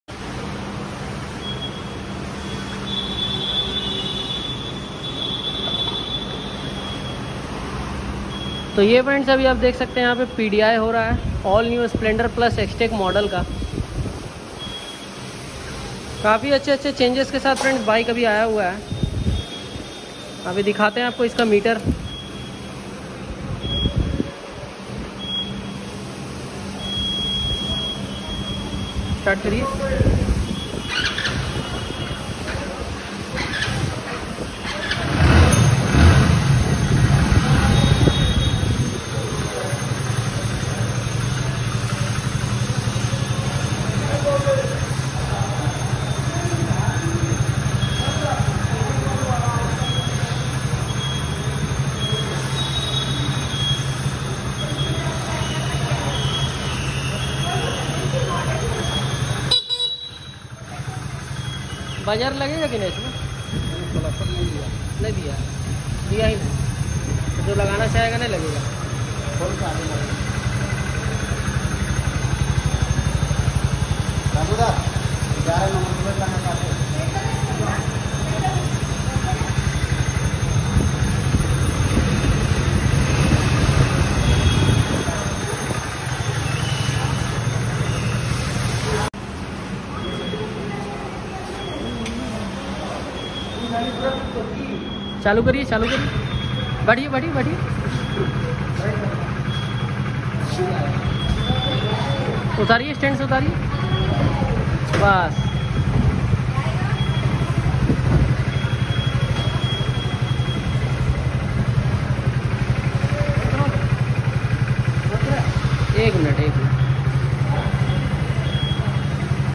New Hero Splendor Plus Xtec 2022 PDI ✔️ | New Battery 😲 | Exhaust Sound 🔥
Hello friends,

In this video I am going to tell you about All new Hero Splendor Plus Xtec BS6 model what's new in this model...?? Complete Detail Review,All New Features👍

hero splendor plus xtec bs6 new colour splendor plus xtec  bs6
new splendor plus xtec
splendor plus xtec
hero
splendor
bs6
bike

so,